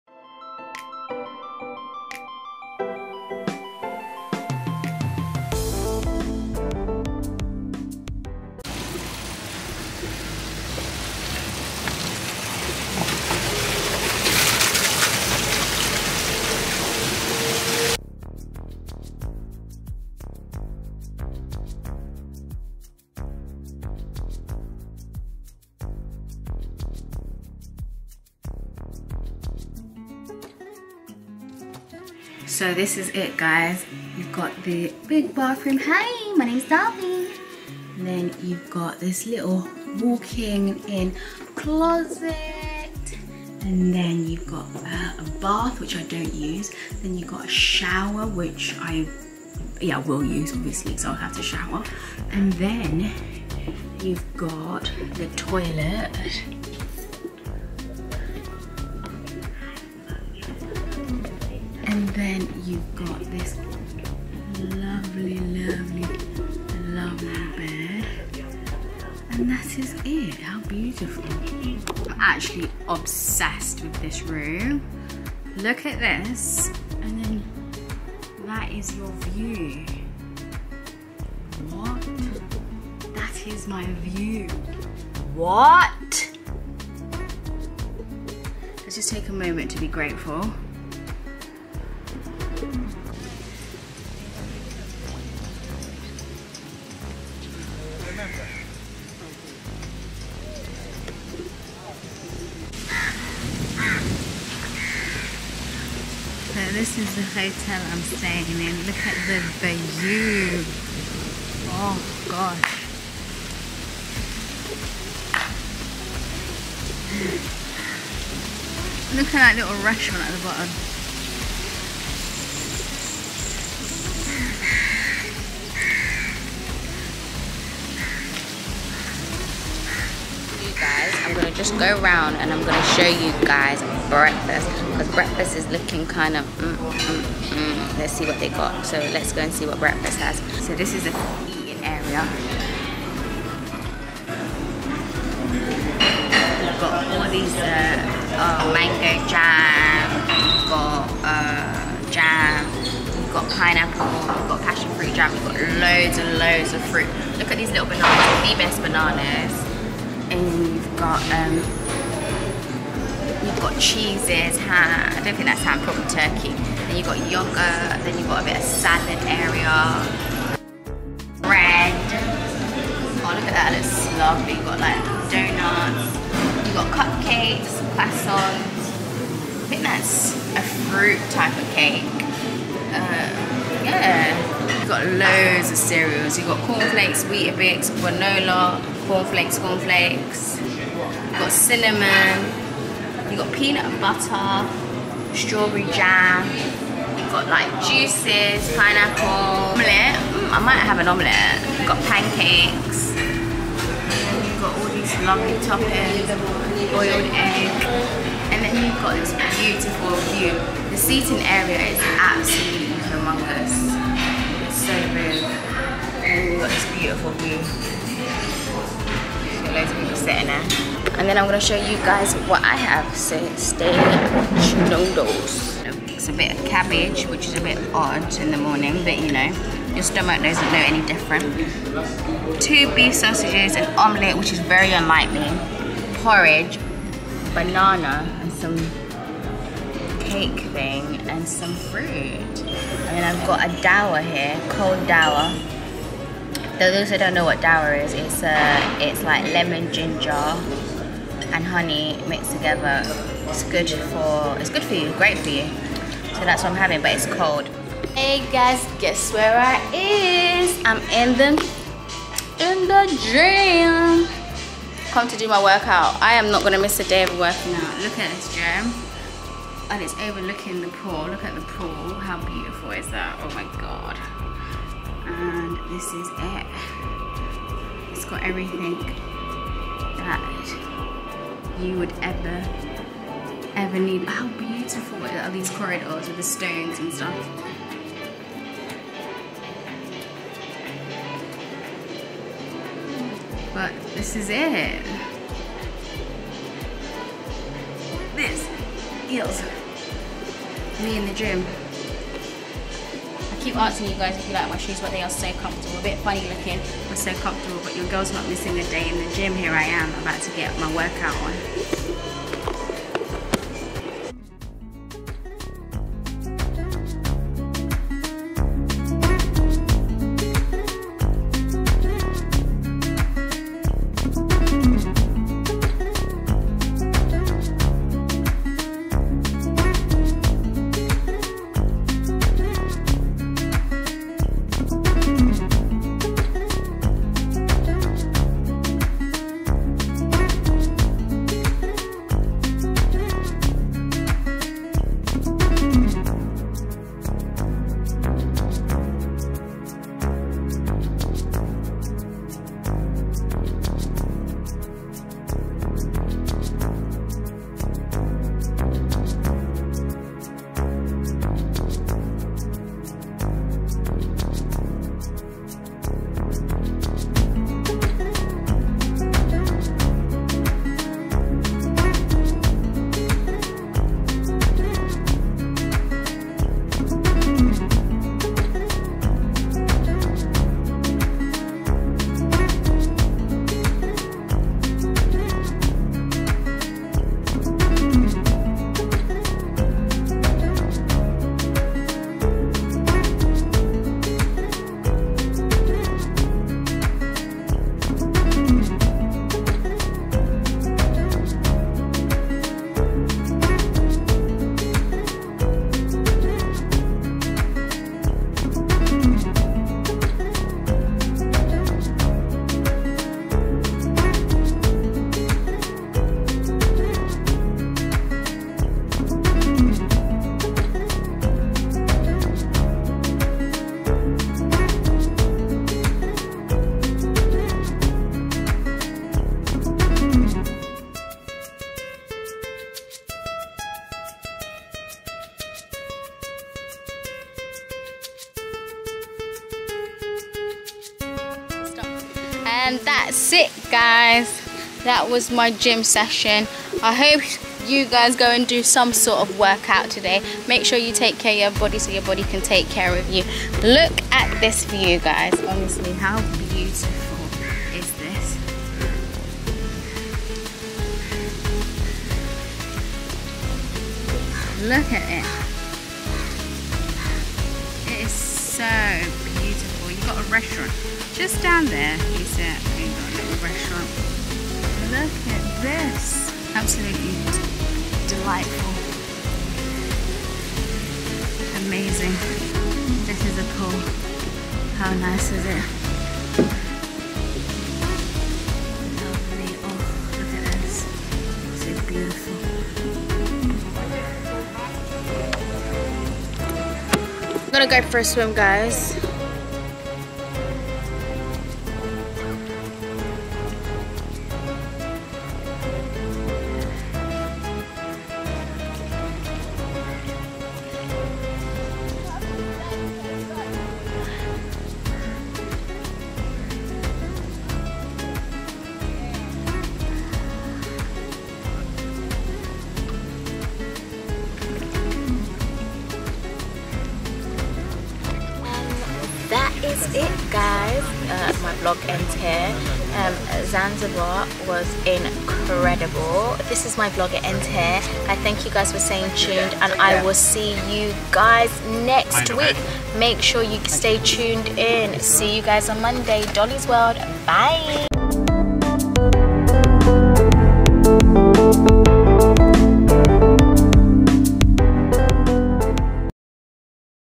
End of Zanzibar trip | Melia Hotel | Zanzibar vlog | Dolly’sworld🌍
Welcome to my Zanzibar vlog! In this video, I'll take you on a tour of the beautiful beaches and crystal clear waters of Zanzibar. You'll get a taste of the delicious local cuisine, including fresh seafood and traditional Swahili dishes.And don't miss the Swahili night event! You'll experience the music, dancing, and colorful costumes of this vibrant culture, and get a chance to meet and connect with locals.